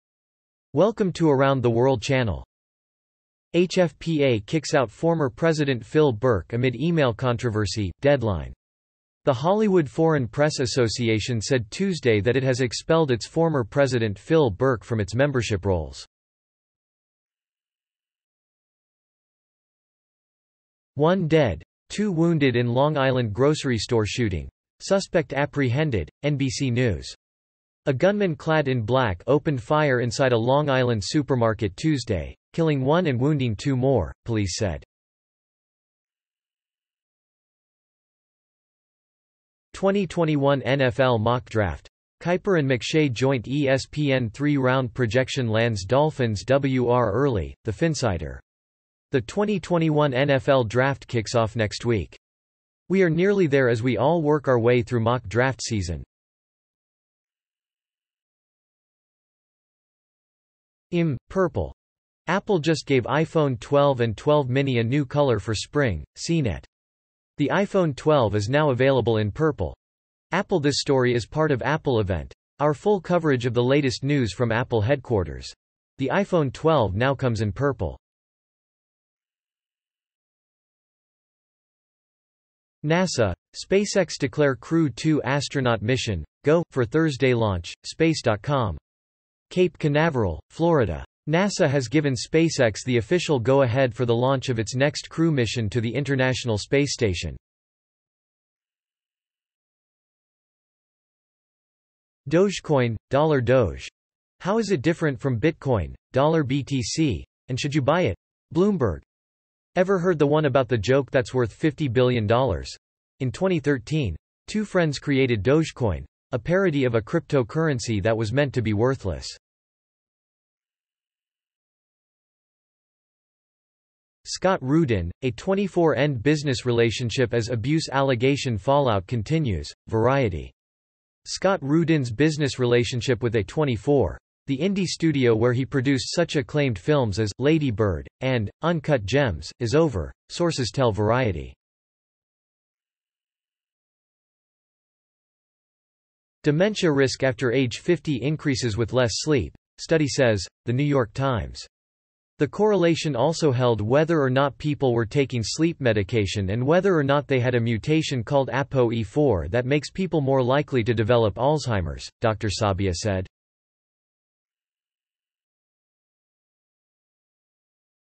HFPA Kicks Out Former President Phil Berk Amid Email Controversy - Deadline
Deadline - Patrick Hipes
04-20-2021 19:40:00
The Hollywood Foreign Press Association said Tuesday that it has expelled its former president Phil Berk from its membership rolls. “Effective immediately, Phil Berk is no longer a member of the Hollywood Foreign Press Association,” the HFPA board said in a s...
The Hollywood Foreign Press Association said Tuesday that it has expelled its former president Phil Berk from its membership rolls.

NBC News - Wilson Wong, Tom Winter, David K. Li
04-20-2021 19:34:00
1 dead, 2 wounded in Long Island grocery store shooting; suspect apprehended - NBC News
The shooting occurred at a Stop & Shop grocery store in West Hempstead, New York, authorities said.
A gunman clad in black opened fire inside a Long Island supermarket Tuesday, killing one and wounding two more, police said.

The Phinsider - Kevin Nogle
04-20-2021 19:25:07
2021 NFL Mock Draft: Kiper and McShay joint-ESPN three-round projection lands Dolphins WR early - The Phinsider
The 2021 NFL Draft kicks off next week. We are nearly there as we all work our way through Mock Draft Season. As we are reaching the culmination of projections, predictions, assumptions, and smoke...
The 2021 NFL Draft kicks off next week. We are nearly there as we all work our way through Mock Draft Season.

CNET - Patrick Holland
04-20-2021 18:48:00
Mmmmm, purple: Apple just gave iPhone 12 and 12 Mini a new color for spring - CNET
Apple announced the new purple iPhone 12 on Tuesday along with new iMacs, an updated AppleTV, AirTags and an iPad Pro.
The iPhone 12 is now available in purple.AppleThis story is part of Apple Event, our full coverage of the latest news from Apple headquarters.The iPhone 12 now comes in purple.

Space.com - Amy Thompson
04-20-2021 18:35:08
NASA, SpaceX declare Crew-2 astronaut mission 'go' for Thursday launch - Space.com
Liftoff is scheduled for Thursday, April 22.
CAPE CANAVERAL, Fla. NASA has given SpaceX the official go-ahead for the launch of its next crew mission to the International Space Station.

Bloomberg - Charlie Wells, Katharine Gemmell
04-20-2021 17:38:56
Dogecoin ($DOGE): How Is It Different From Bitcoin ($BTC) and Should You Buy It? - Bloomberg
Should investors consider backing a joke?
Ever heard the one about the joke thats worth $50 billion?In 2013, two friends created Dogecoin - a parody of a cryptocurrency that was meant to be worthless.

Variety - Brent Lang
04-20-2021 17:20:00
Scott Rudin, A24 End Business Relationship as Abuse Allegation Fallout Continues - Variety
Popular on Variety Scott Rudins business relationship with A24, the indie studio where he produced such acclaimed films as “Lady Bird” and “Uncut Gems,” is over, sources tell Variety. The severing of ties comes in the wake of allegations of workplace abuses ...
Scott Rudins business relationship with A24, the indie studio where he produced such acclaimed films as “Lady Bird” and “Uncut Gems,” is over, sources tell Variety.

New York Times - Pam Belluck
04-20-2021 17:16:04
Dementia Risk After Age 50 Increases With Less Sleep, Study Says - The New York Times
The research, tracking thousands of people from age 50 on, suggests those who sleep six hours or less a night are more likely to develop dementia in their late 70s.
The correlation also held whether or not people were taking sleep medication and whether or not they had a mutation called ApoE4 that makes people more likely to develop Alzheimers, Dr. Sabia said.

This channel and all content is made automatically by a robot. For futher knowledge and/or suggestions, please get in touch with us.